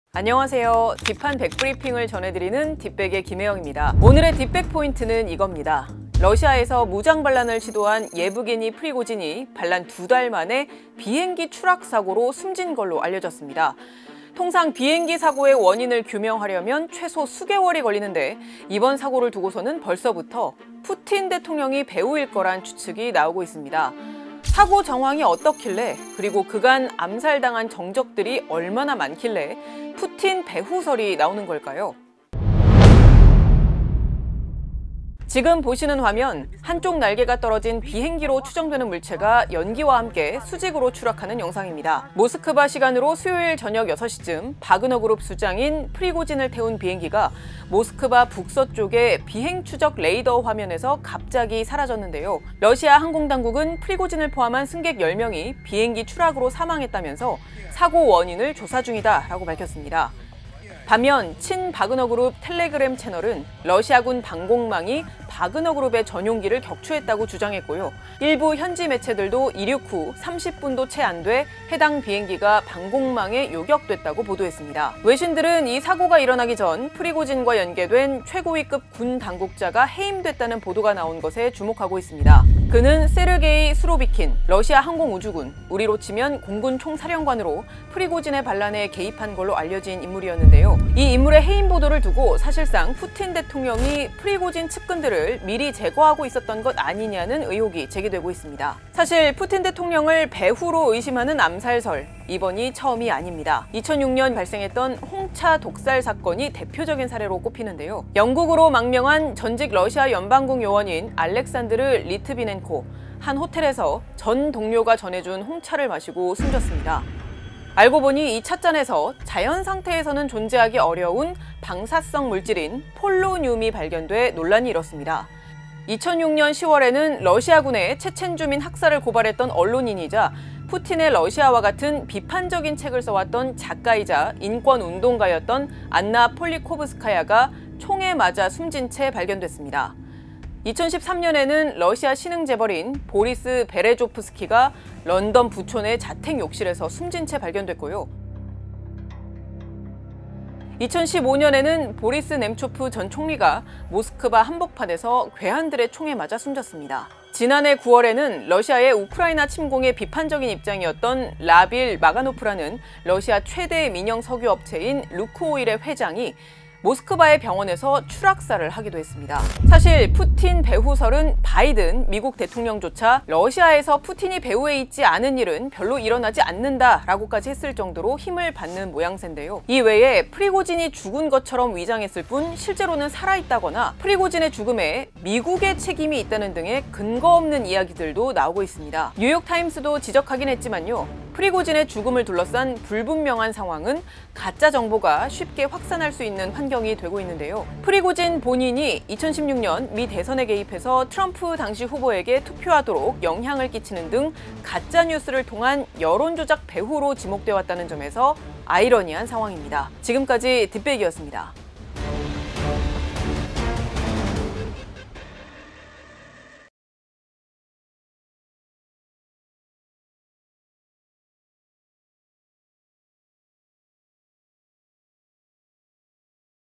푸틴 눈 밖에 나는 족족 사망...프리고진 사망은 사고일까 암살일까? / SBS /
러시아에서 무장 반란을 시도한 예브게니 프리고진이 반란 두 달 만에 비행기 추락 사고로 숨졌습니다. 통상 비행기 사고의 원인을 규명하려면 최소 수 개월이 걸리는데, 이번 사고를 두고서는 벌써부터 푸틴 대통령이 배후일 거란 추측이 나오고 있습니다. 대체 사고 정황이 어떻길래, 그간 암살 당한 정적들이 얼마나 많길래 '푸틴 배후설'이 계속 나오는 걸까요? '딥한 백브리핑 : 딥빽'에서 자세히 알아봤습니다.

취재·구성 : 김혜영 기자, 영상취재 : 김태훈, 영상편집 : 한만길, CG : 장지혜, 작가 : 서정민, 인턴 : 오소영, 기획 : 디지털뉴스제작부